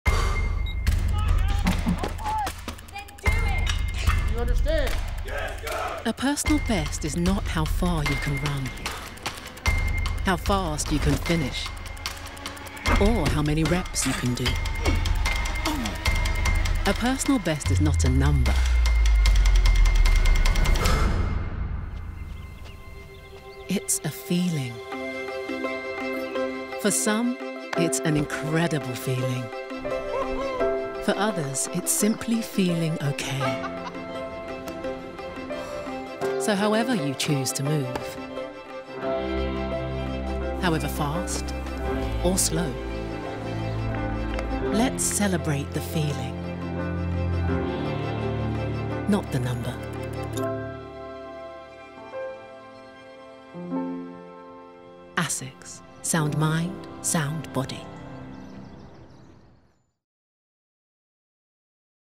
ASICS | A personal best is not a number. It's a feeling.
Join us this World Mental Health Day to redefine a personal best to focus on how exercise makes us feel. Every image you share with NewPersonalBest will help us raise funds for mental health charities. ​

Research shows that millions of people are missing out on the benefits of exercise because they find sport and exercise intimidating.​

That’s why we’re championing a more welcoming exercise culture that celebrates the way exercise makes us all feel. No times. No stats. No numbers. ​

We are ASICS. ‘Anima Sana In Corpore Sano’, a Sound Mind in a Sound Body. ​